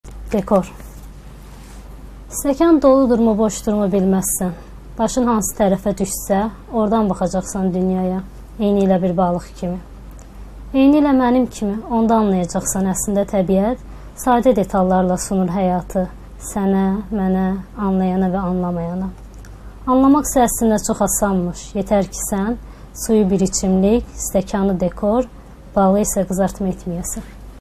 Dekor
Aytən Nəsibbəyli Azadlıq Radiosunun OxuZalı üçün şeirini oxuyur